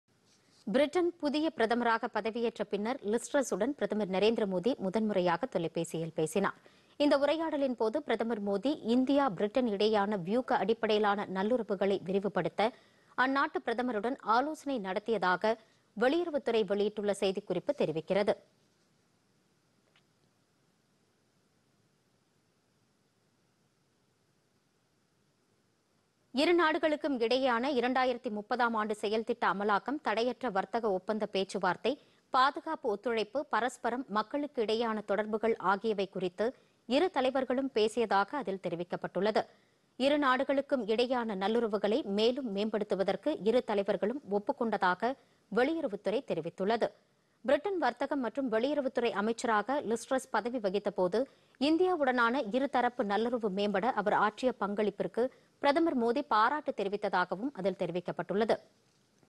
பிரிட்டன் பிரதமருடன் நரேந்திர மோதி பேச்சுPodhigaiTamilNews
பிரிட்டன் பிரதமர் லிஸ் ட்ரஸ்-வுடன்   பிரதமர் நரேந்திர மோதி தொலைபேசியில் பேச்சு -  இந்தியா - பிரிட்டன் இடையேயான நல்லுறவுகளை மேம்படுத்த முடிவு.